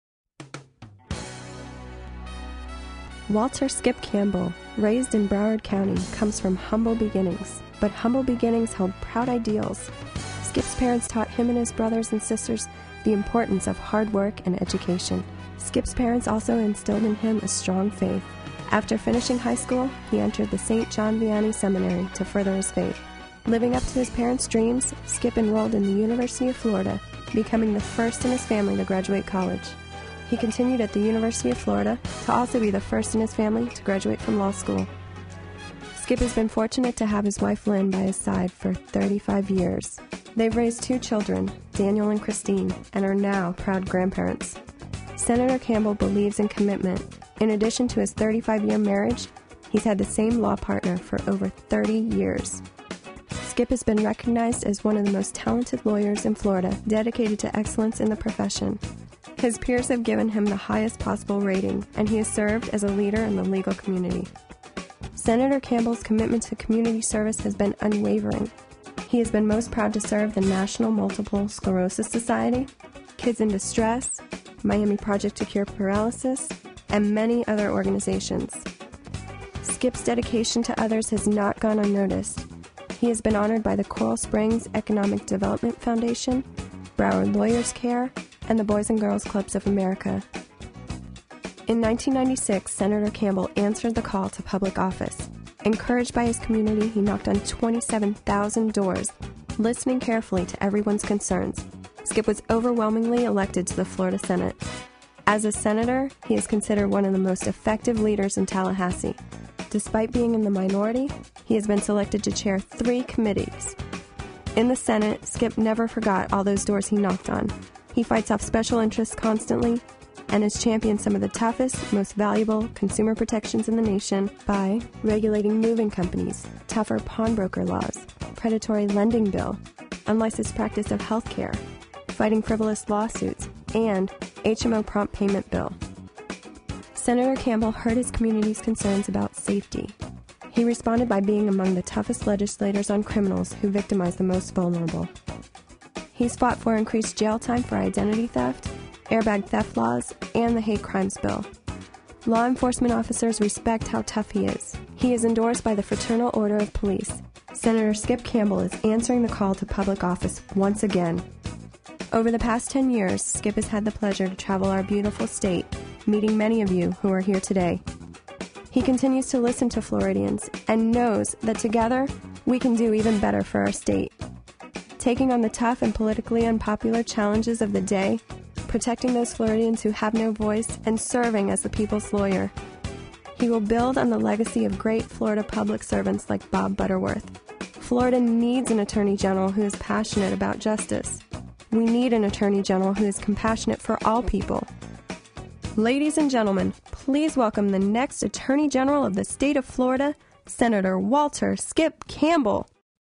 Introduction Video for Senator Walter Skip Campbell
Video used to introduce senator Walter Skip Campbel when he was in campaign going for Florida's General Attorney position.